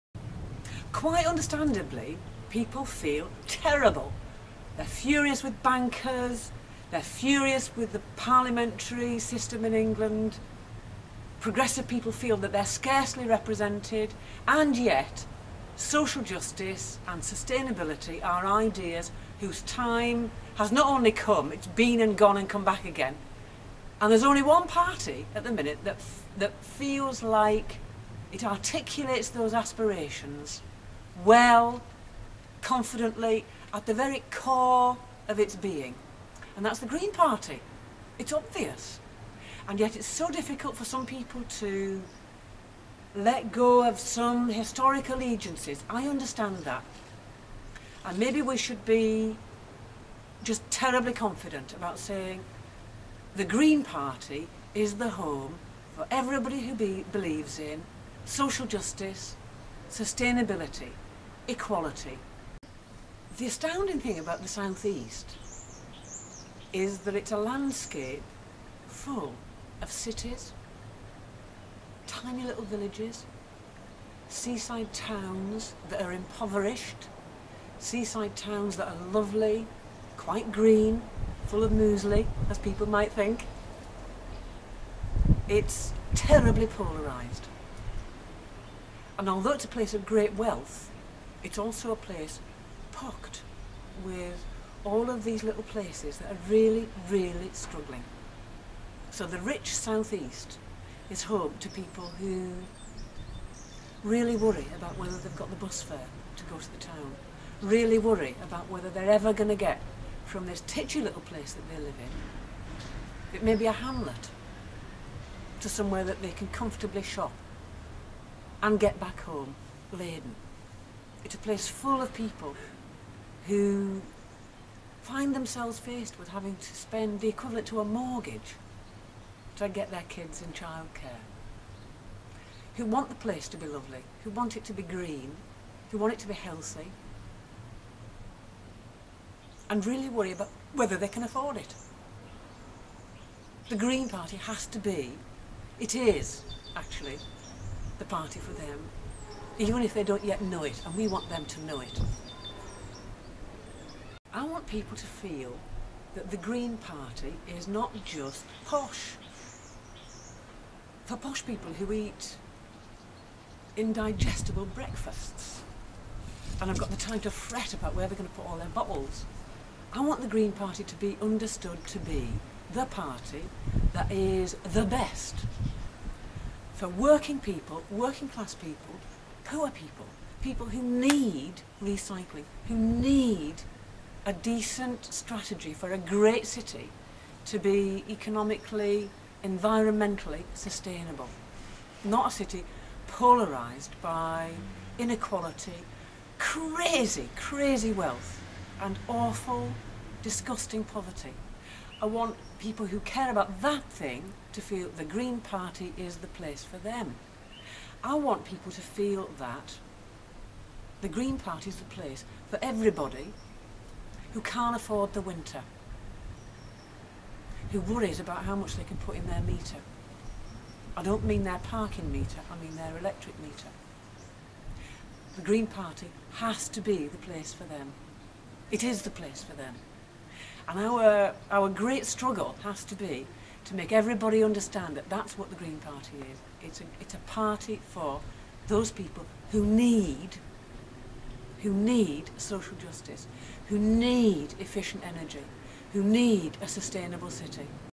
Beatrix Campbell
Beatrix campbell speaks about why she thinks the Green Party is where it is at.